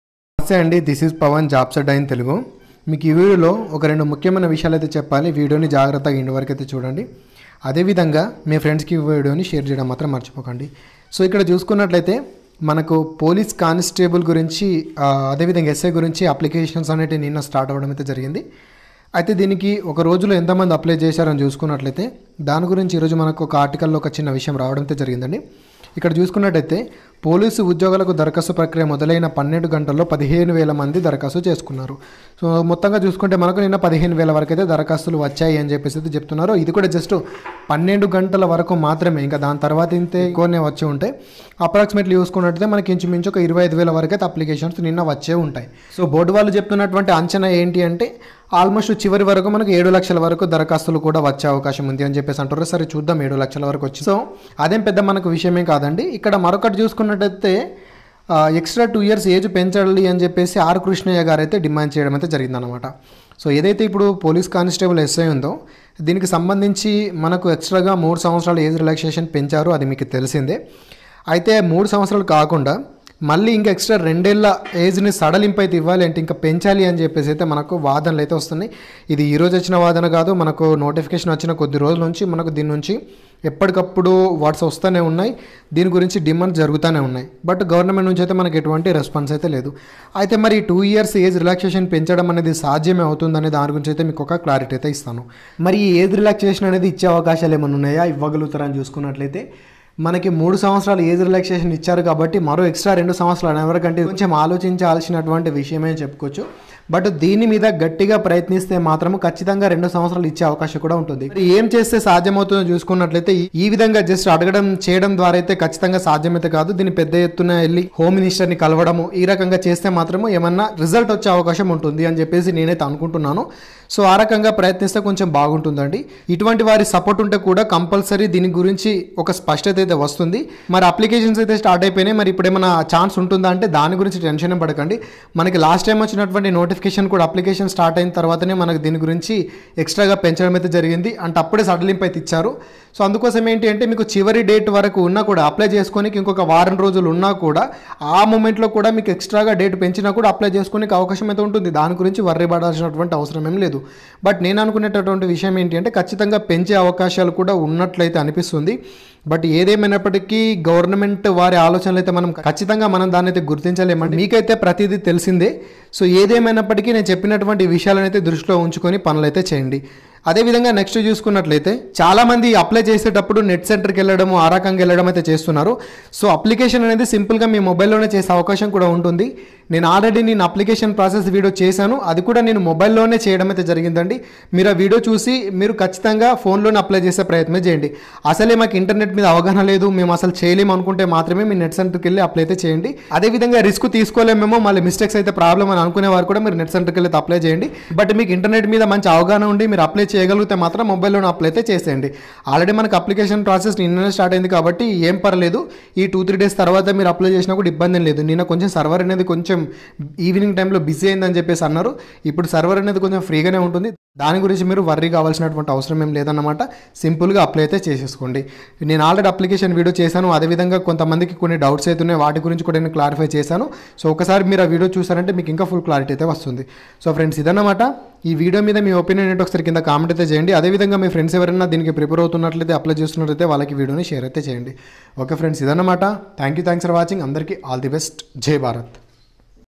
TS Police ఎన్ని దరఖాస్తులు | Extra 2 Years Age Relaxation ఇస్తారా | TS Police Apply in Mobile
Ts jobs, telangana job notifications, ts police jobs,tslprb telangana state police constable recruitment 2022, ts police,